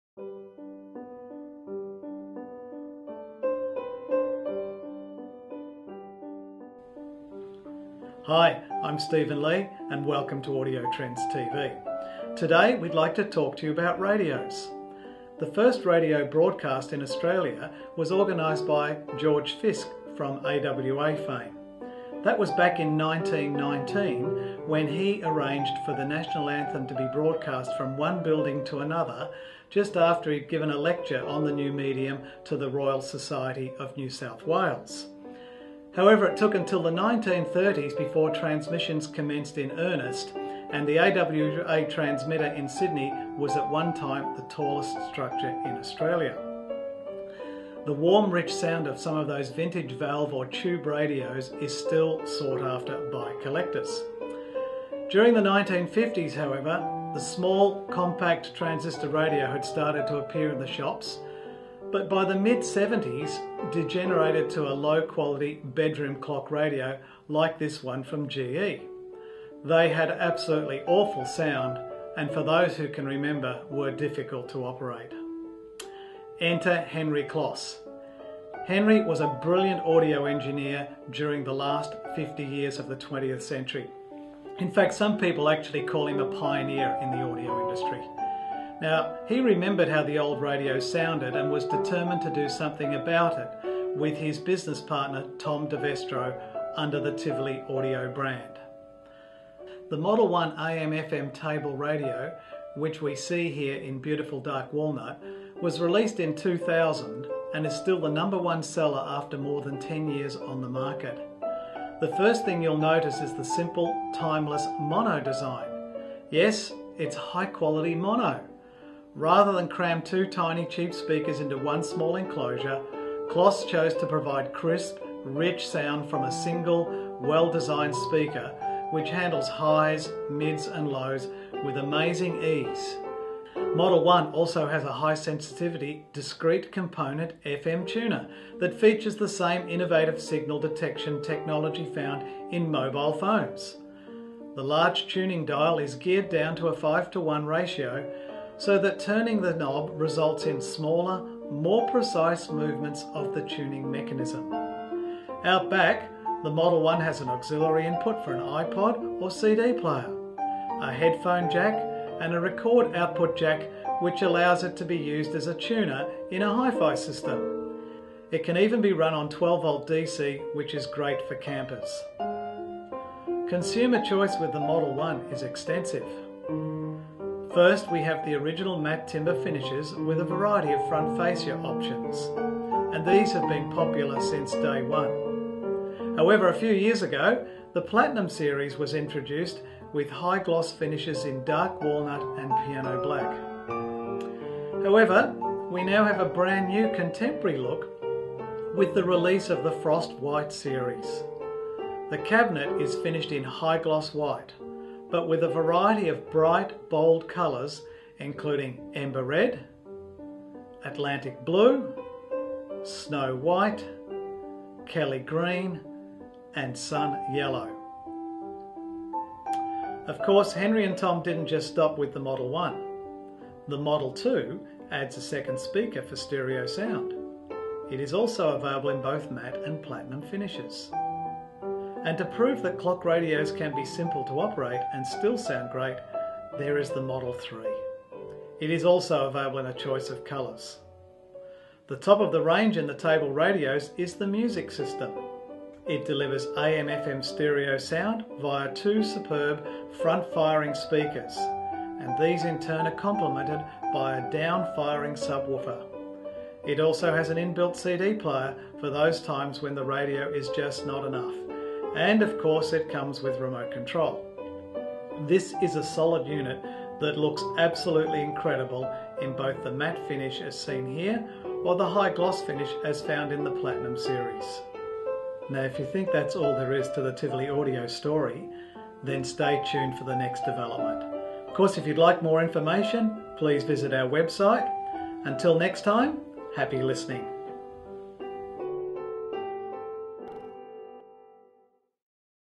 Tivoli Audio Table Radios - Why Are They So Good?
Want to know more about Tivoli Audio Radios? Audio Trends Company Founder who has been selling these fantastic radios almost since day 1, drills down on some of the different models and features.Check out for more information and latest deals.